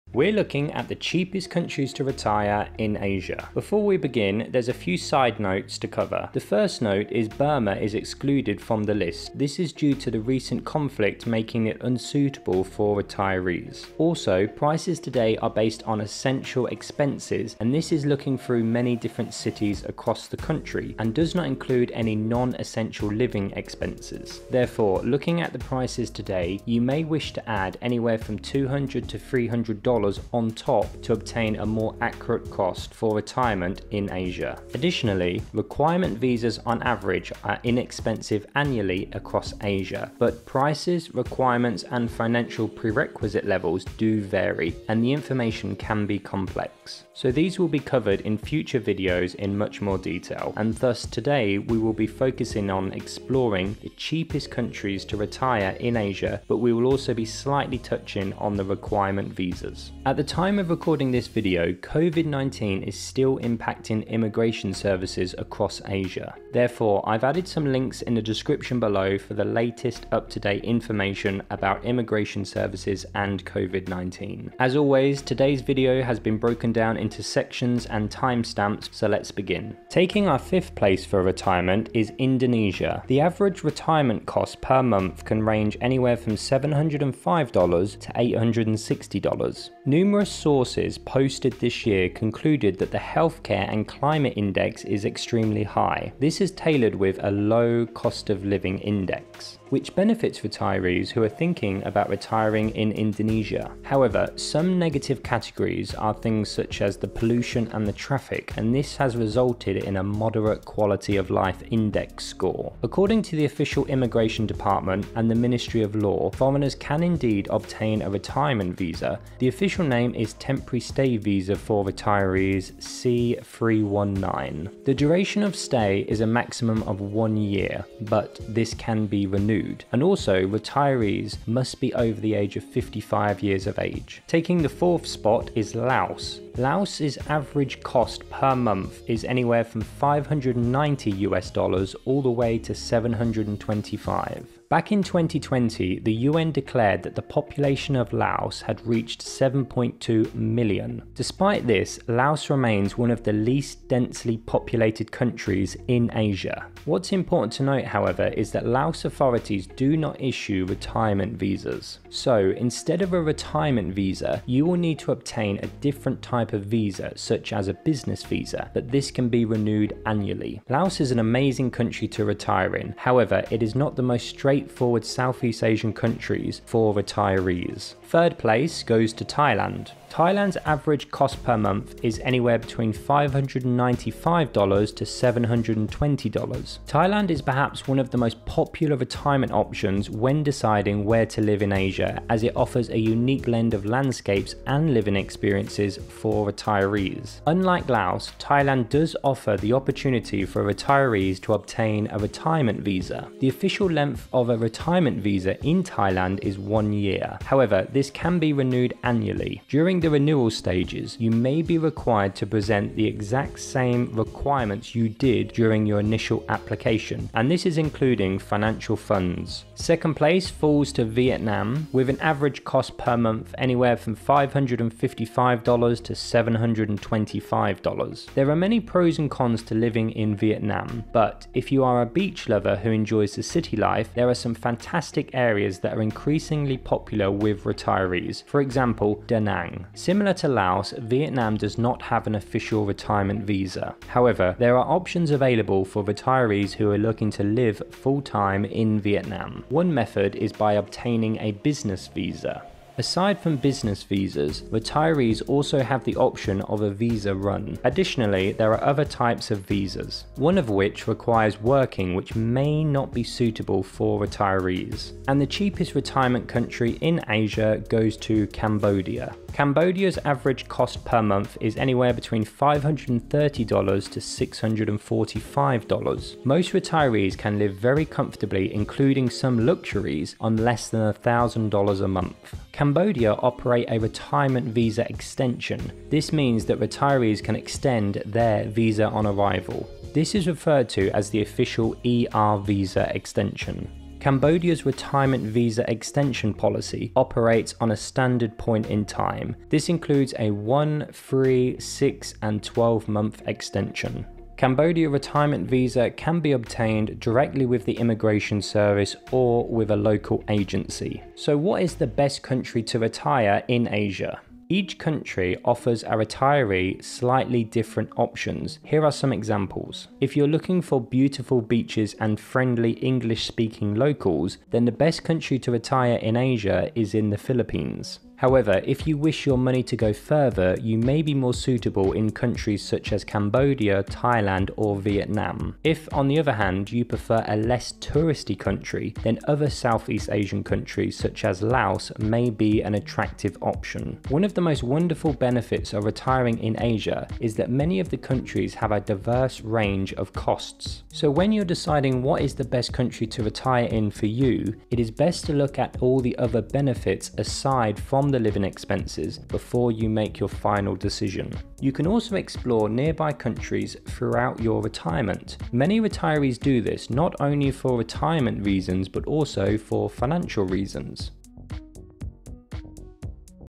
Cheapest Country To Retire In Asia - Retire in Asia - Retirement In Asia Cheap - Retirement Visa
Cheapest Country To Retire In Asia: At the time of retiring, there are many exciting decisions to make. One of those decisions is what place to retire in. For many retiring in Asia is a lifelong dream, and thus, we will be exploring today the cheapest countries to retire in Asia.

Before we begin, there are many different options when looking at the cheapest countries to retire in Asia. But one statistically proven fact is that the cheapest countries to retire in Asia are situated in Southeast Asia. Furthermore, the data and feedback used to calculate the list of cheap Asian countries to retire specifically cover basic living expenses for retirees but do not include unessential or luxurious costs.

TIMESTAMPS

00:00 - Introduction
01:20 - 5th Place
02:14 - 4th Place
02:57 - 3rd Place
03:40 - 2nd Place
04:24 - 1st Place
05:09 - What Is The Best Country To Retire In Asia

What Is The Best Country To Retire In Asia?
When determining the best country for retirement in Asia, one has to consider one’s lifestyle expectations. Each country offers a retiree slightly different options. Here are some examples.

If you are looking for beautiful beaches and friendly locals, the best country to retire in Asia is the Philippines. However, if you wish your money to go further, you will be most suitable in countries such as Cambodia or Thailand. If you prefer a less touristy country than other southeast Asian countries, Laos is an attractive option.

Today we explored the cheapest countries to retire in Asia, and thus it is highly likely that you are looking for your money to go further. Still, one of the most wonderful benefits of retiring in Asia is that many countries have a diverse range of costs. So when deciding what the best country to retire in is, it is best to look at other benefits aside from living expenses before making your final decision.

One last benefit that is sometimes overlooked when retiring in Asia cheaply is that you have the opportunity to explore nearby countries throughout your retirement. Many retirees do this not only for retirement reasons but also for financial reasons.